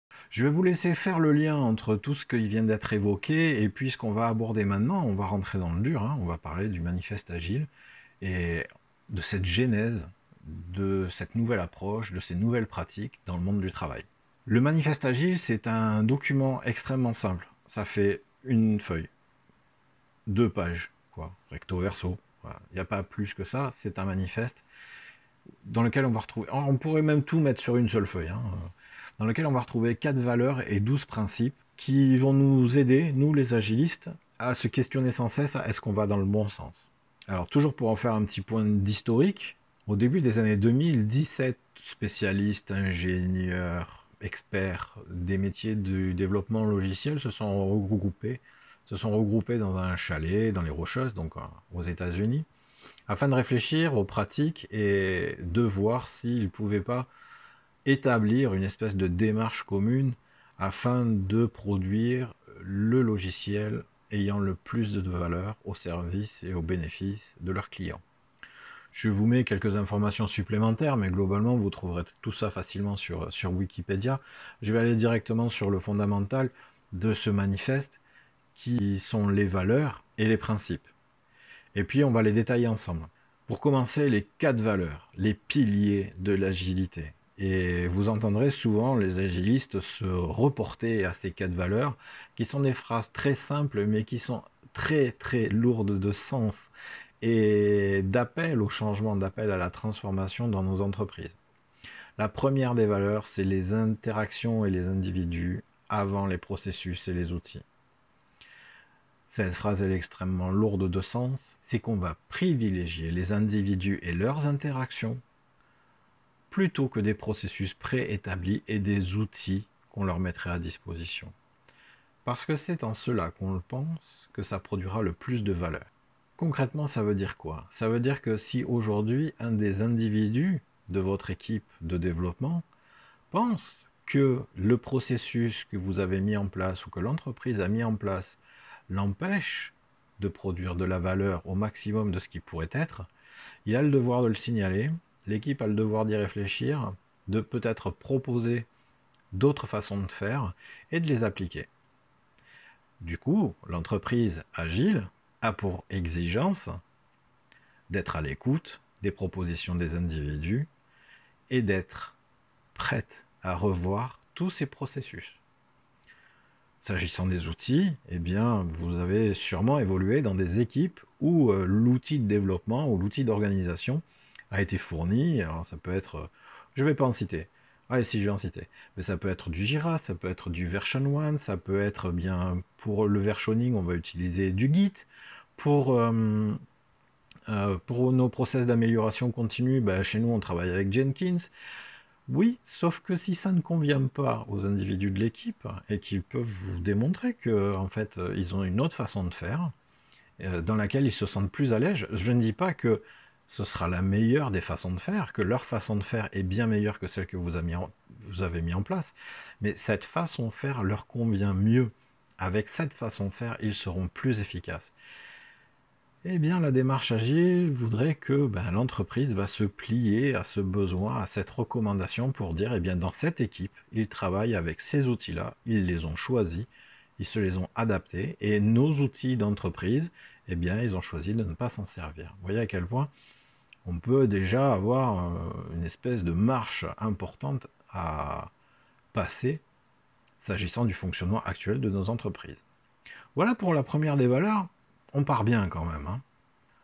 Le manifeste Agile et la première des valeurs
Cette vidéo est issue de ma formation Scrum que je libère aujourd'hui pour que chacun puisse en profiter gratuitement.